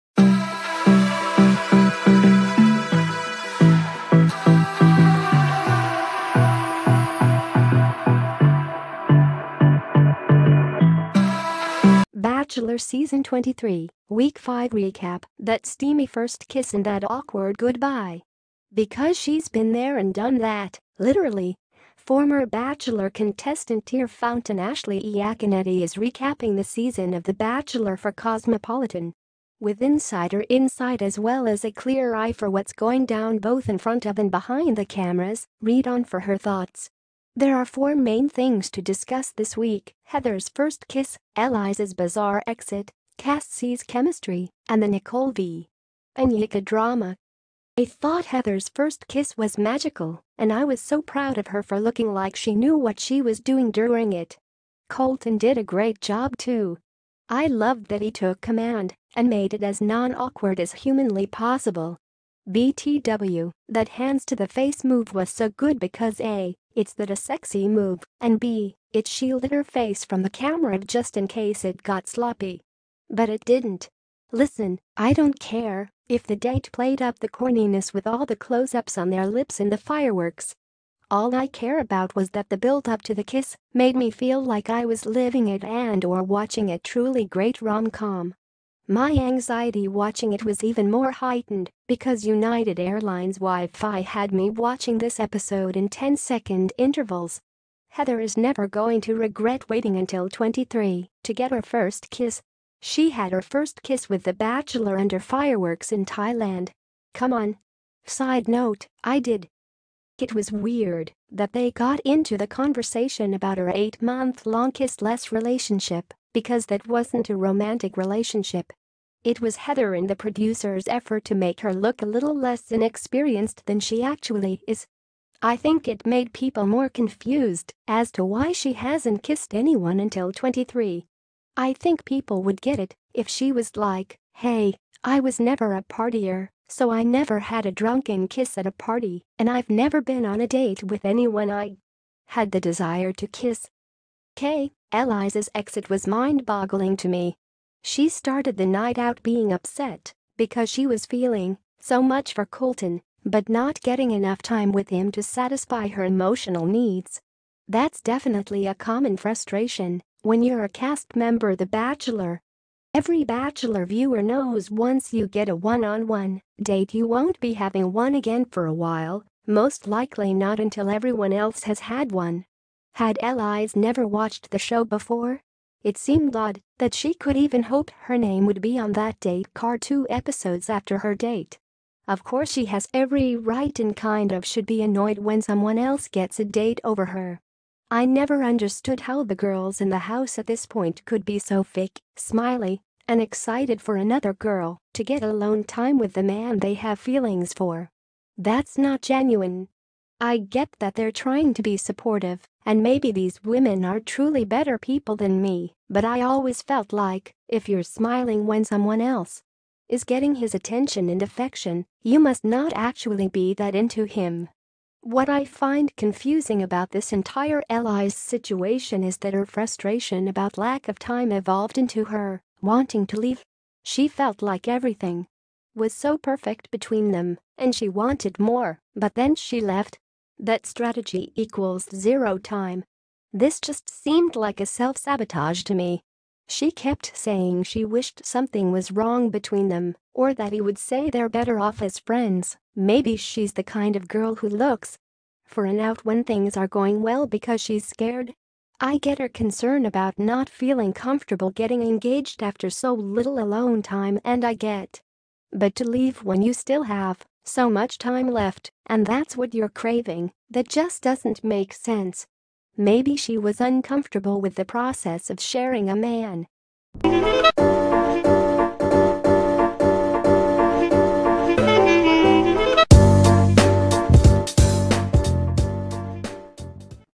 'Bachelor' Season 23, Week 5 Recap: That Steamy First Kiss and That Awkward Goodbye
'Bachelor' Season 23, Week 5 Recap: That Steamy First Kiss and That Awkward Goodbye. Because she's been there and done that, literally, former Bachelor contestant/tear fountain Ashley Iaconetti is recapping this season of The Bachelor for Cosmopolitan. With insider insight as well as a clear eye for what's going down both in front of and behind the cameras, read on for her thoughts.  There are four main things to discuss this week: Heather’s first kiss, Elyse’s bizarre exit, Cassie’s chemistry, and the Nicole v. Onyeka drama. I thought Heather’s first kiss was magical and I was so proud of her for looking like she knew what she was doing during it. Colton did a great job, too. I love that he took command and made it as non-awkward as humanly possible. BTW, that hands to the face move was so good because a) it’s that a sexy move, and b) it shielded her face from the camera just in case it got sloppy. But it didn’t!  Listen, I don’t care if the date played up the corniness with all the close-ups on their lips and the fireworks. All I care about was that the build-up to the kiss made me feel like I was living it and/or watching a truly great rom-com. My anxiety watching it was even more heightened because United Airline’s Wi-Fi had me watching this episode in ten second intervals.  Heather is never going to regret waiting until 23 to get her first kiss! She had her first kiss with THE BACHELOR UNDER FIREWORKS IN THAILAND! COME ON! Side note: I did think it was weird that they got into the conversation about her eight-month long “kiss-less” relationship, because that wasn’t a romantic relationship … that was a friendship. It was Heather and the producers’ effort to make her look a little less inexperienced than she actually is.    I think it made people more confused as to why she hasn’t kissed anyone until 23. I think people would get it if she was like, “Hey, I was never a